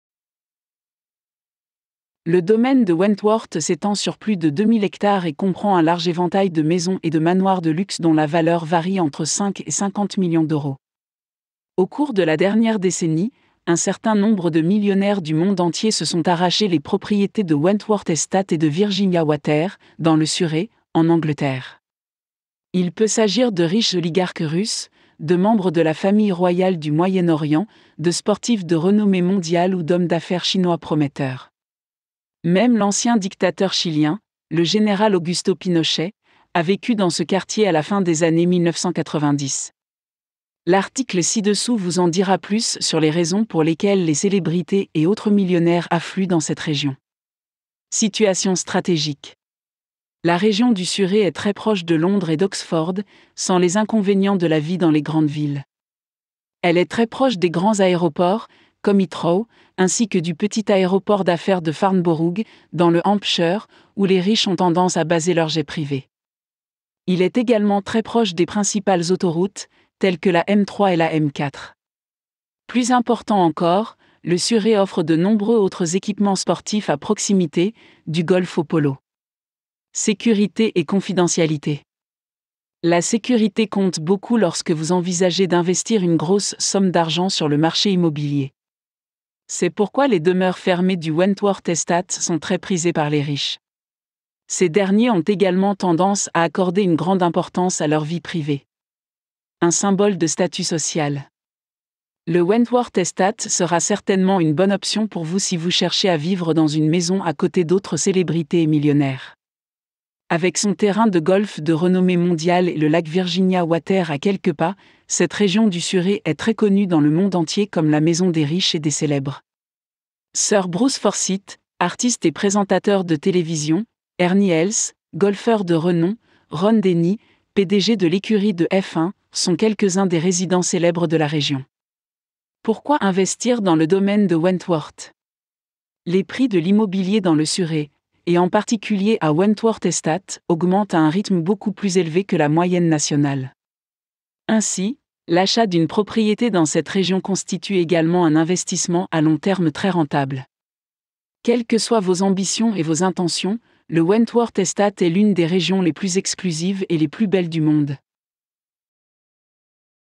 Pourquoi les célébrités et les millionnaires affluent à Wentworth Estate
Construisez des textes, des articles de blog sans écrire une seule ligne:
Pourquoi les célébrités et les millionnaires affluent vers le domaine de Wentworth ; Le domaine de Wentworth s'étend sur 2 000 acres et comprend un large éventail de maisons et de villas de luxe, évaluées entre 5 et 50 millions de livres sterling.     Au cours de la dernière décennie, un certain nombre de millionnaires du monde entier se sont emparés des propriétés de Wentworth Estate et de Virginia Water à Surrey, en Angleterre. Il peut s'agir de riches oligarques russes, de membres de la famille royale du Moyen-Orient, d'athlètes de renommée mondiale ou d'hommes d'affaires chinois prometteurs. Même l'ancien dictateur chilien, le général Augusto Pinochet, vivait dans ce quartier à la fin des années 1990.     Lisez l'article ci-dessous pour en savoir plus sur les raisons pour lesquelles les célébrités et autres millionnaires affluent dans cette région.     Situation stratégique Le quartier du Surrey est très proche de Londres et d'Oxford sans les inconvénients de vivre dans les grandes villes. Il est très proche des grands aéroports tels que Heathrow, ainsi que Farnborough, le petit aéroport d'affaires du Hampshire, où les riches garent leurs jets privés.     Il est également très proche des principales autoroutes telles que la M3 et la M4.     Plus important encore, Surrey a de nombreux autres sports à proximité, du golf au polo.     Sécurité et confidentialité La sécurité est un facteur important lorsque vous envisagez d'investir beaucoup d'argent sur le marché immobilier. C'est pourquoi les maisons de Wentworth Estate sont si populaires auprès des riches. Ils attachent également une grande importance à leur intimité.     Un symbole de statut Le domaine de Wentworth est certainement une excellente option pour vous si vous souhaitez vivre dans une maison avec d'autres célébrités et millionnaires.     Avec son parcours de golf de renommée mondiale et le lac Virginia Water à quelques pas, cette partie de Surrey est connue dans le monde entier comme la patrie des riches et des célébrités. Sir Bruce Forsyth (artiste et présentateur de télévision), Ernie Els (golfeur célèbre), Ron Dennis (PDG de l'équipe de F1) sont parmi les résidents les plus célèbres de la région.     Pourquoi investir dans le domaine Wentworth ?     Les prix des maisons à Surrey (et dans le domaine de Wentworth en particulier) augmentent beaucoup plus rapidement que la moyenne nationale. Acheter un bien immobilier dans cette région est donc un investissement très intéressant sur le long terme. Quelles que soient vos ambitions et vos intentions, le domaine de Wentworth est l'un des quartiers les plus exclusifs et les plus beaux du monde.